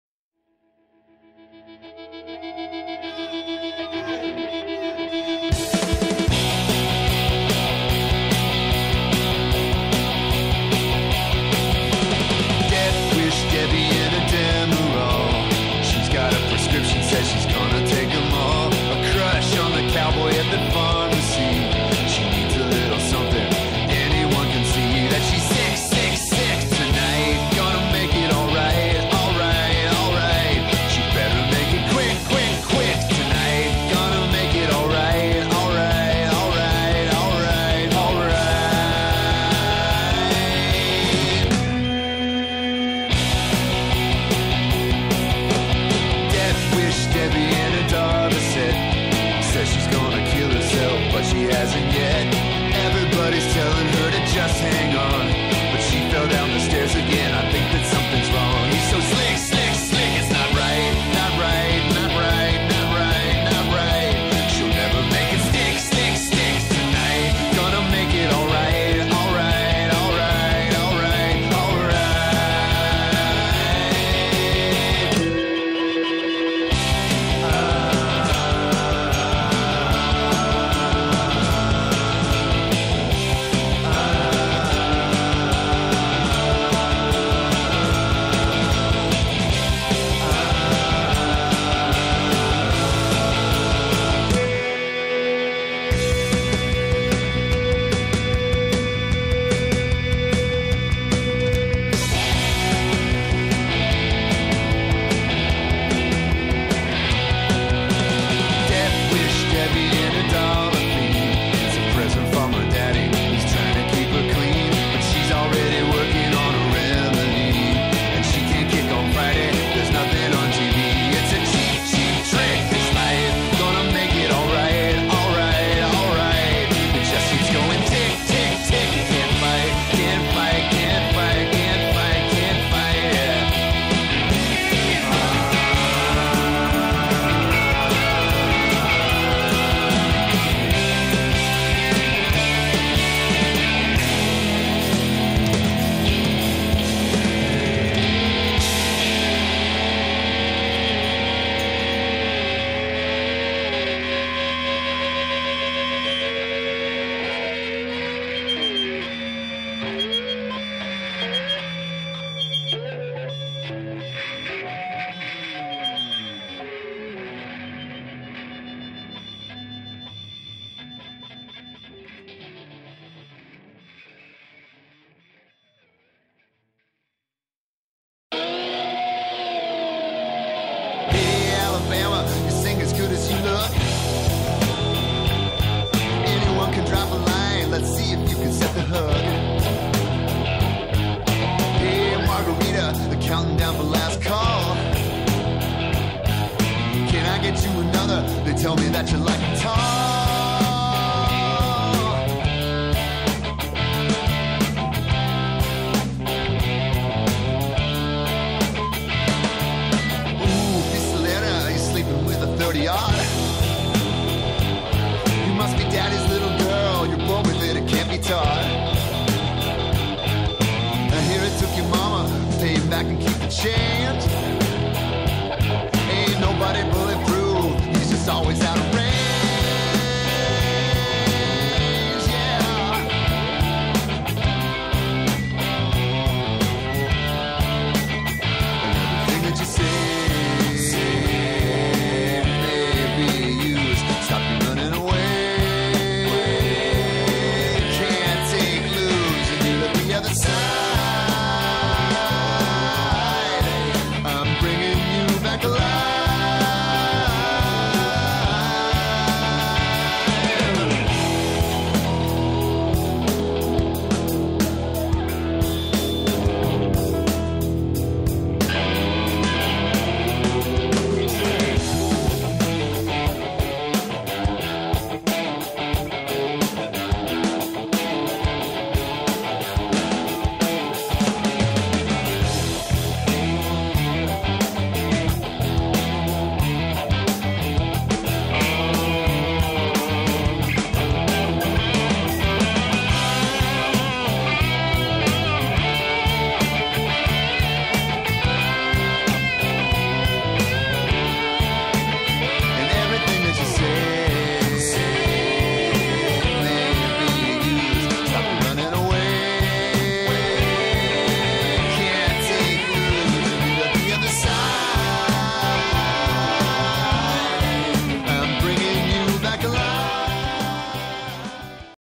Leaf gilding (gold leaf) of a gastank - Tank vergolden
Schnelles und sicheres aufbringen von Blattgold. Mit Schablonen und der Airbrush bzw. Spritzpistole geht alles etwas schneller.

Leaf gilding of a gastank, fast and safe application of gold leaf, with airbrush and masks.